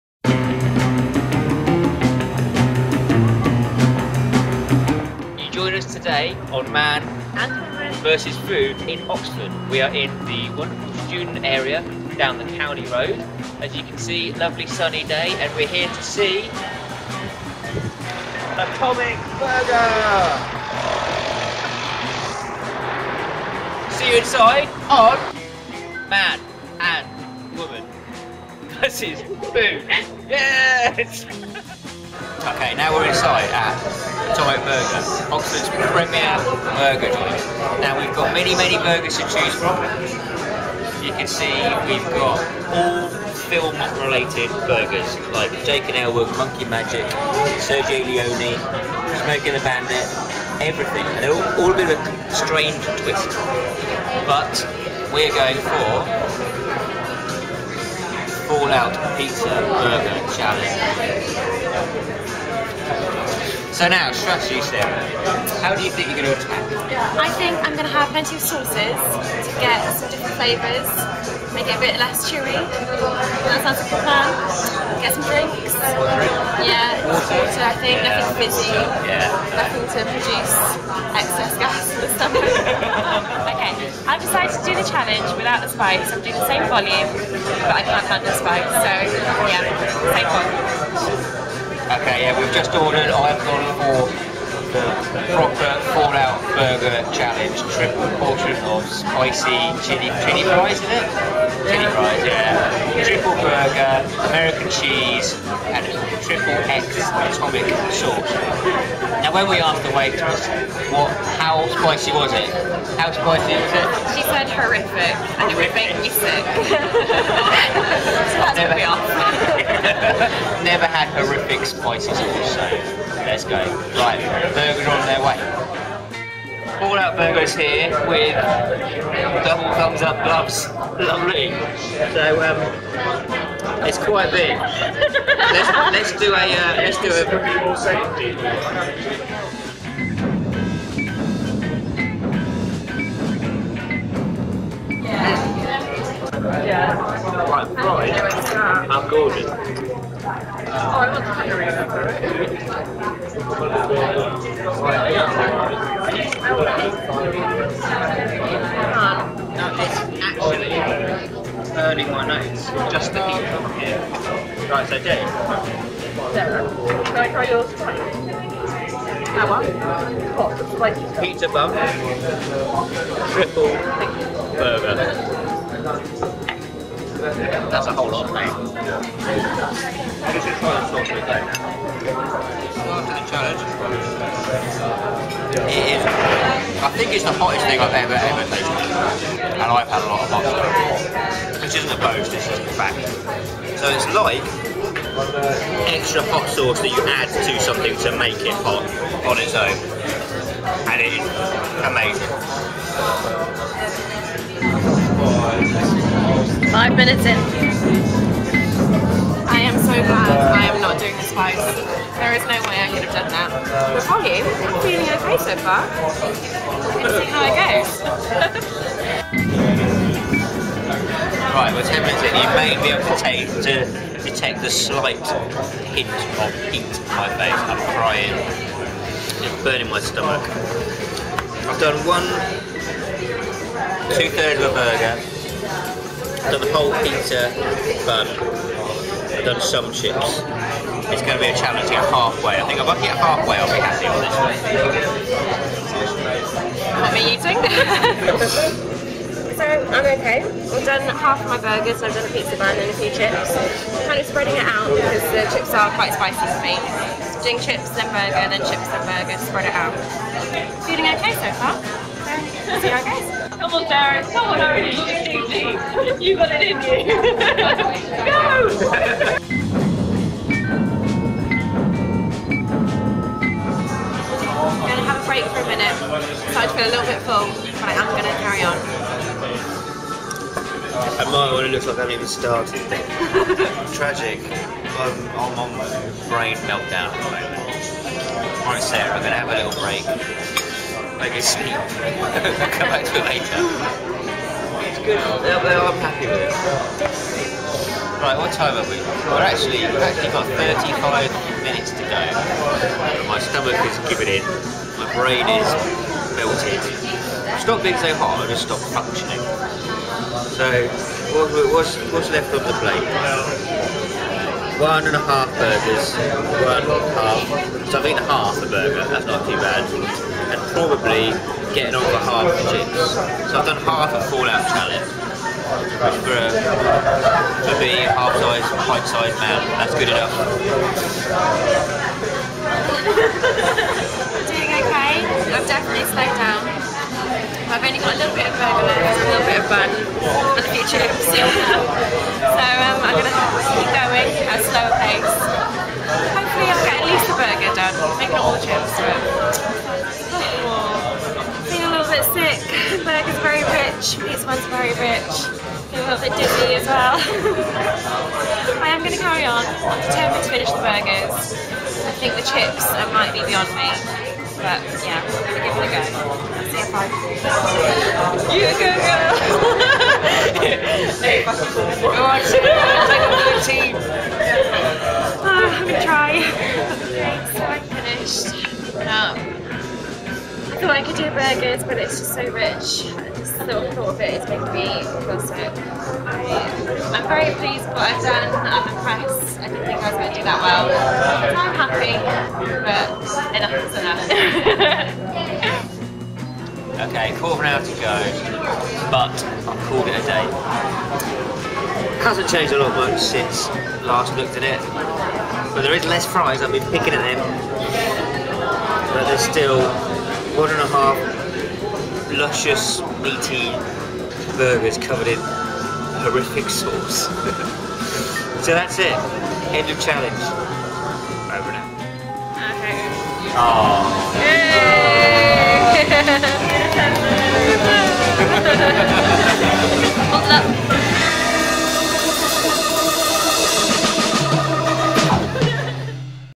Atomic Burger Fallout Pizza Burger Challenge
Sarah and Owen attempt the Atomic Burger Fallout Pizza Burger Challenge.  Will they succeed?